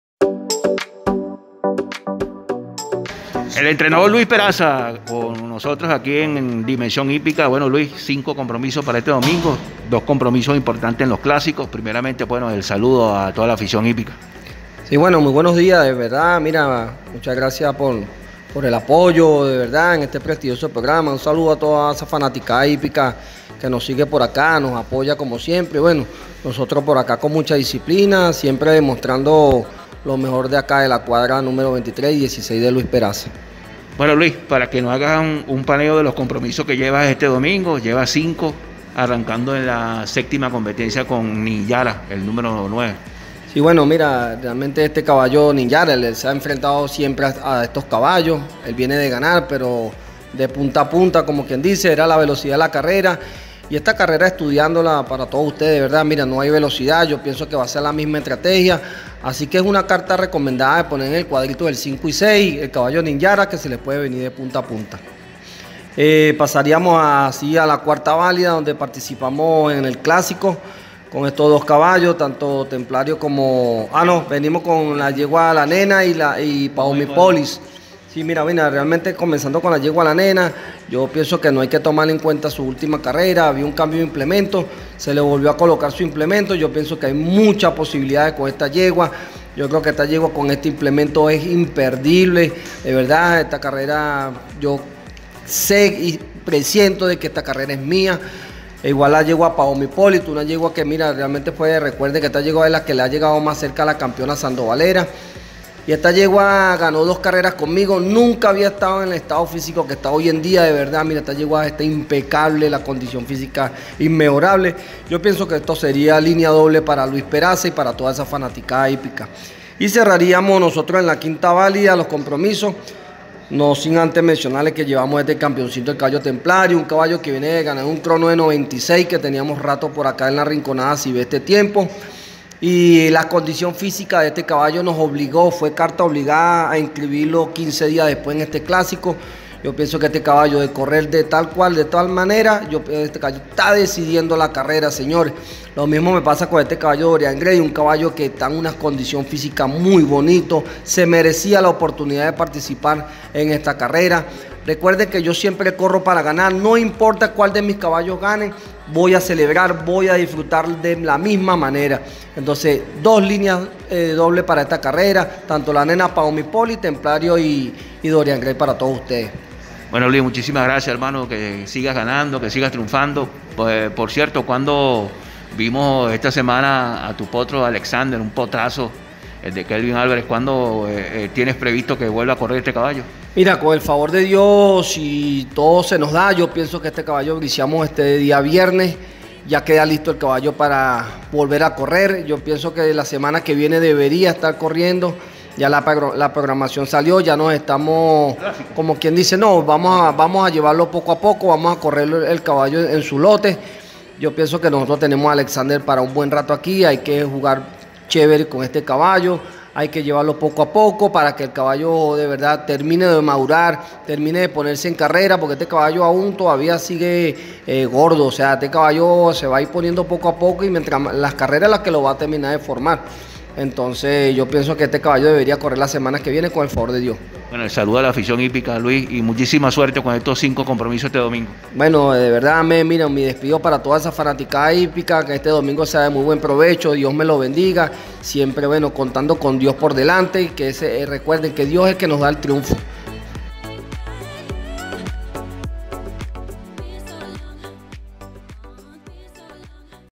Entrevista con el entrenador Luis Peraza / LaRinconada 7-4-2022
El entrenador Luis Peraza ensillará a cinco purasangres este domingo en el Hipódromo La Rinconada y, además, estará presente en los clásicos Peggy Azqueta y Federico Carmona, respectivamente.

Entrevistado por el periodista Antonio José Medina. Caracas, Venezuela.